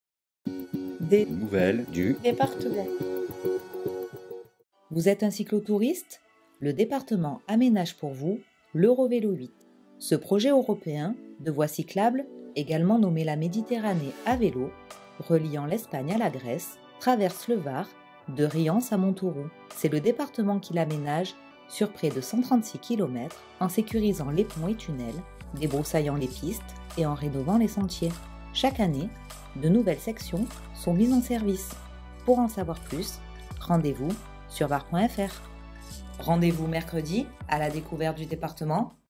Vous êtes un cyclotouriste ? Le Département aménage pour vous l’Eurovélo 8
Ce projet européen de voies cyclables également nommé “La Méditerranée à vélo”, reliant l’Espagne à la Grèce, traverse le Var de Rians à Montauroux.
C’est le Département qui l'aménage sur près de 136 km en sécurisant les ponts et tunnels, débroussaillant les pistes et en rénovant les sentiers.
Chaque année, de nouvelles sections sont mises en service.
Pour en savoir plus, rendez-vous sur var.fr